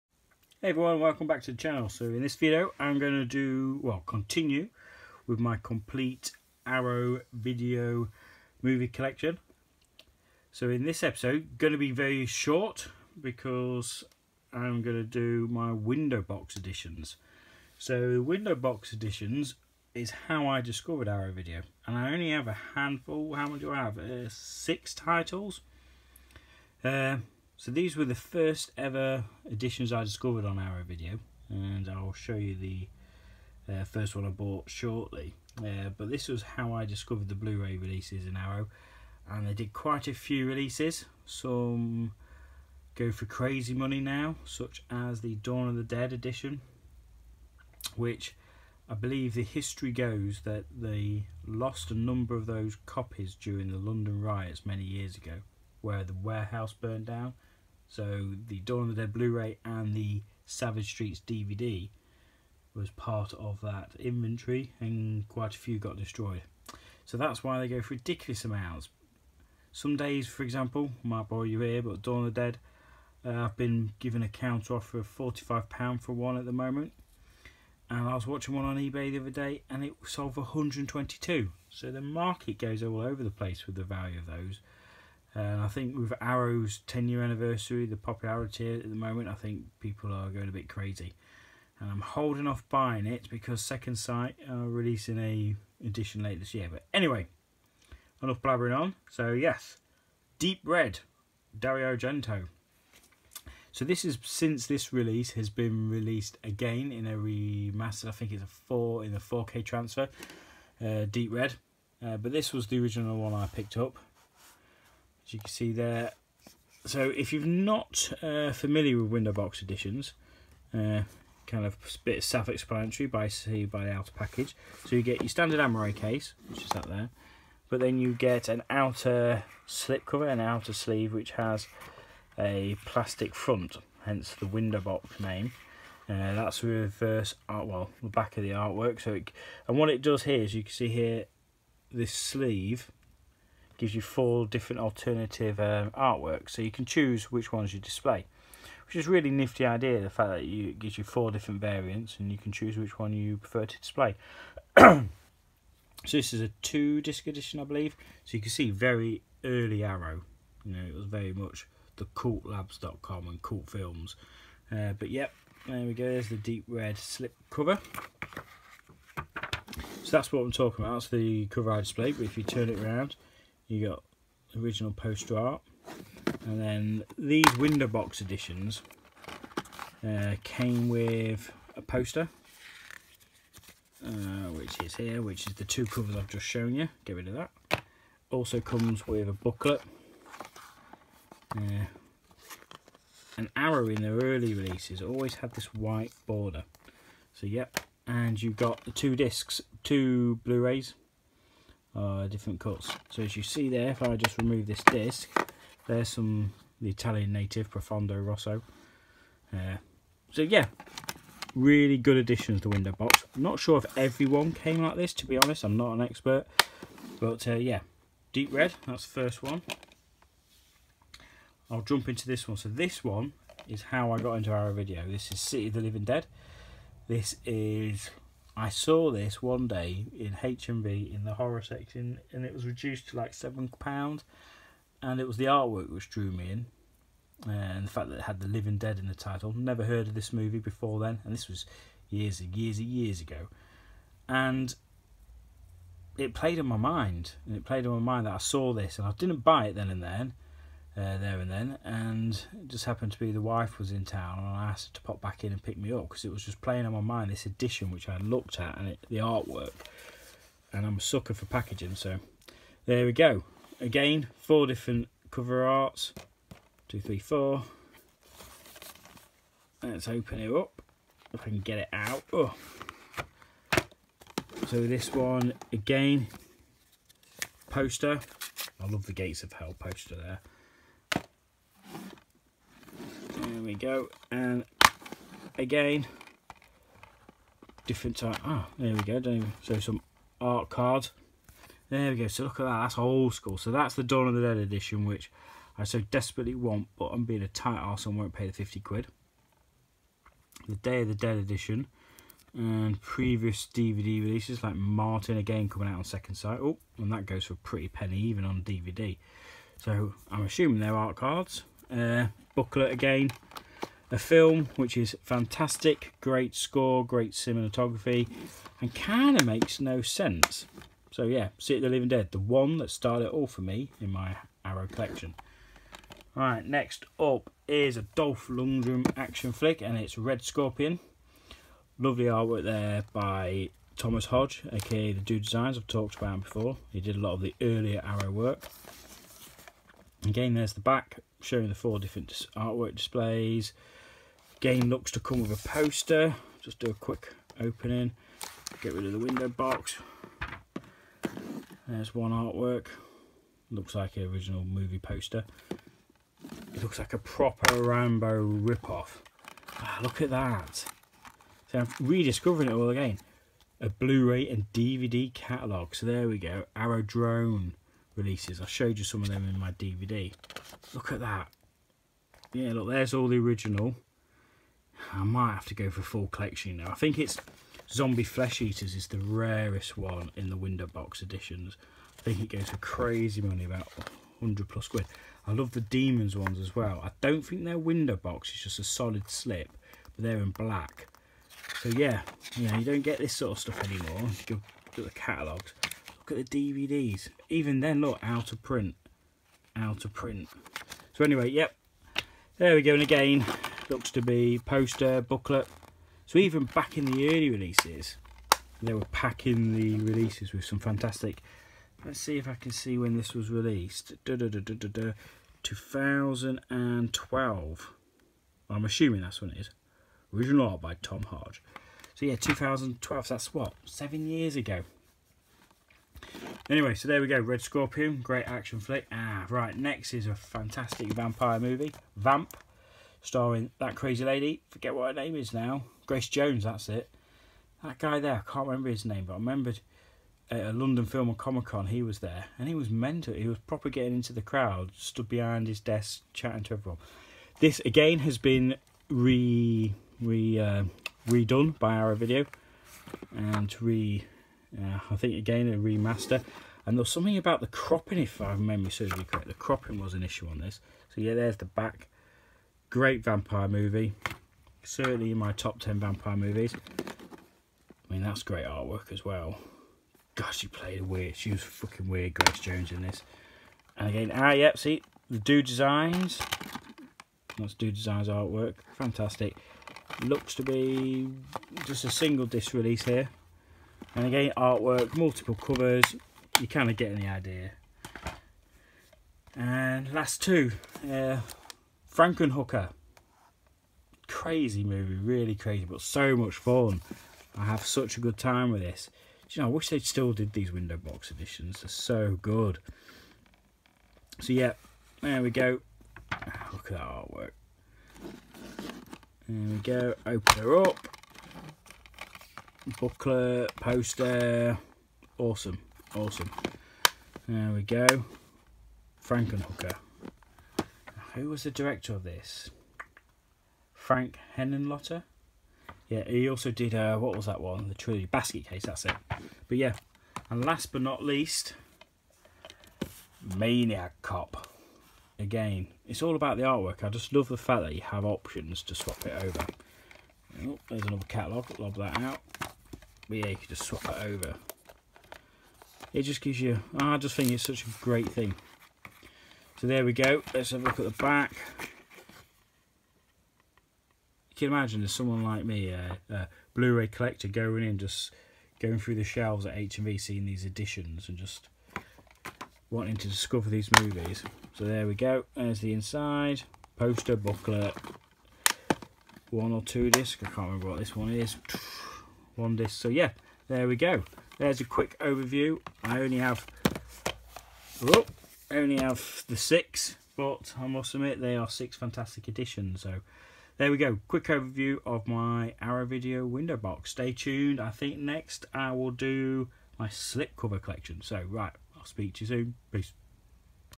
Arrow Video Collection - Window Box Editions
Back with part 3 of the overview of my complete arrow video collection